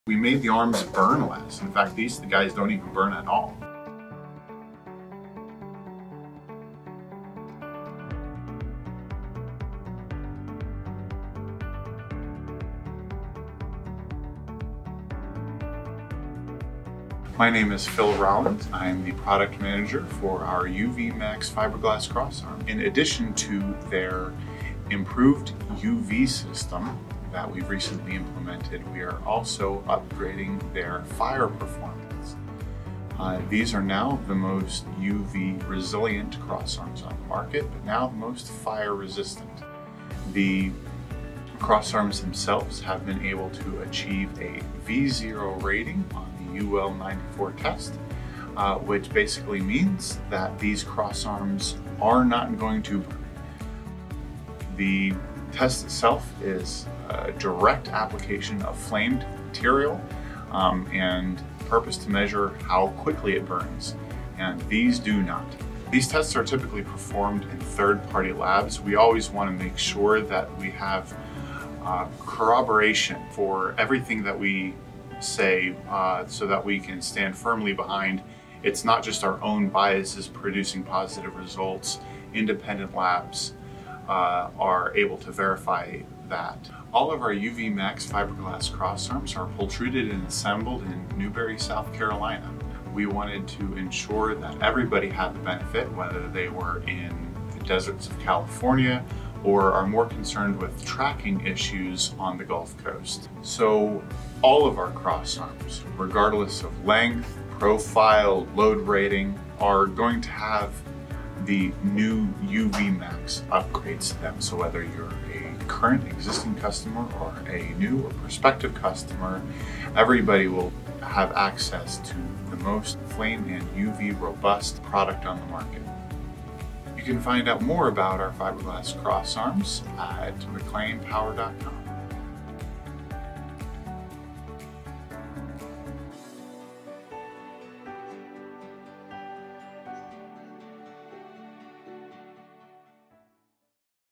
UVMAXX™ Fiberglass Crossarms - UL94 Fire Resiliency | MacLean Power Systems
Introduction to UL94 fire resiliency rating with Product Manger Phill Rollins

To learn more about our distribution, substation, and transmission products, for more information!